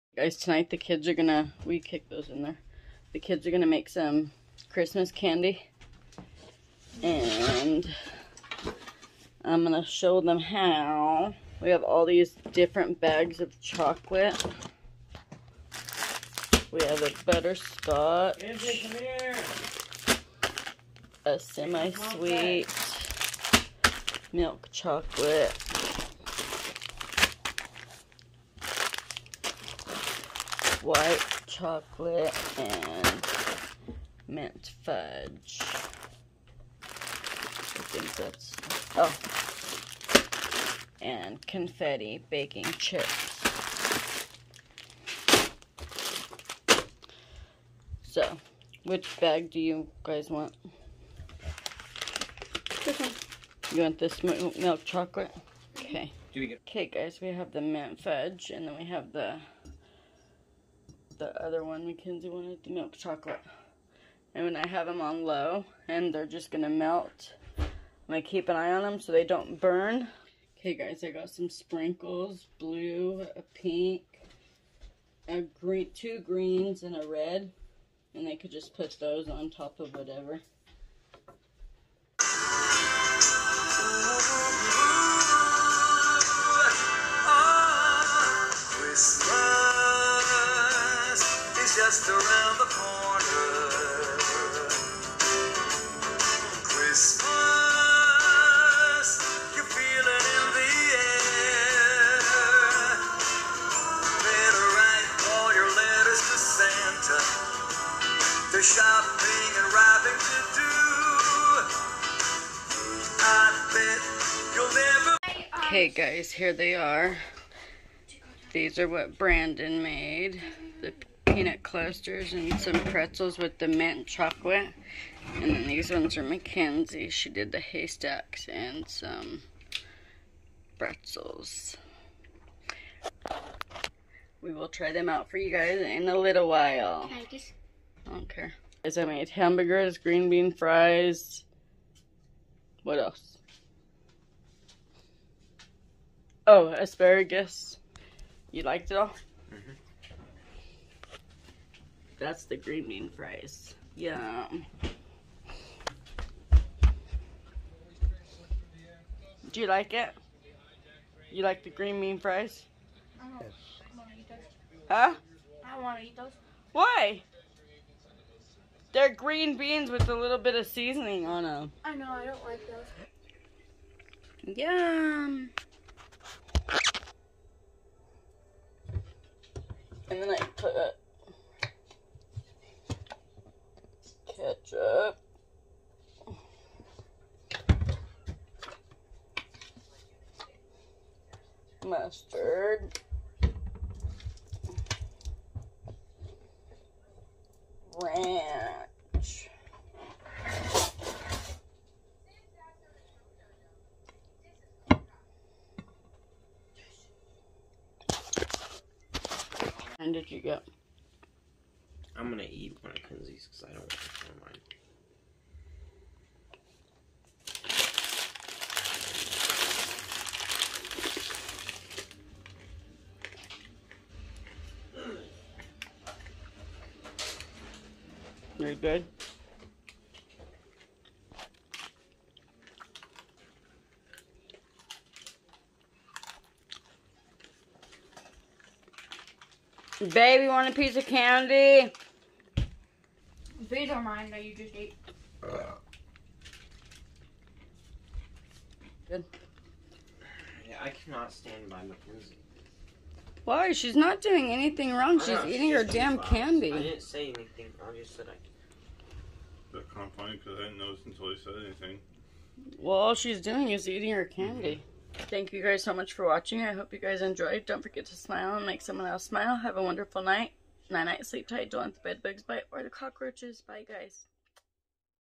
vlogmas Day 20, We maDe Christmas canDy
we made chocolate covered pretzels peanut clusters and haystacks and I make hamburgers green bean fries and asparagus for supper Don't forget to smile and make someone else smile have a great Day and night night sleep tight Don't let the bedbugs bite or the cockroaches...